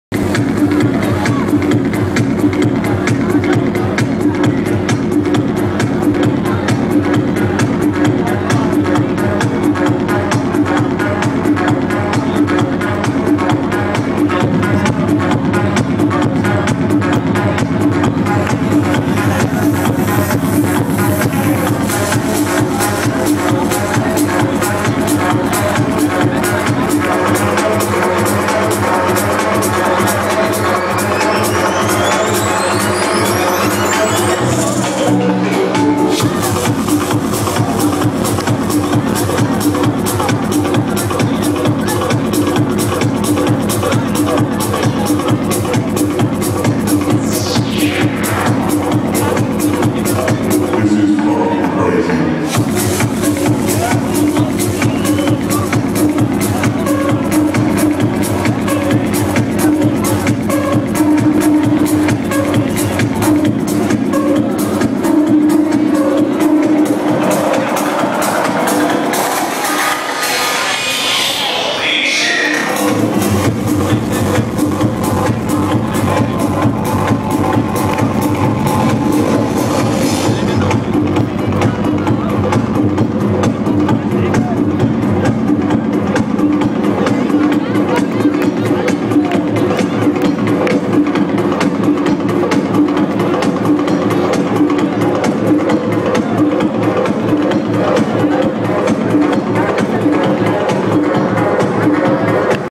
Soenda Indoor Techno Lasershow - 2.MOV
Great techno music during a lasershow at Soenda Indoor @Central Studios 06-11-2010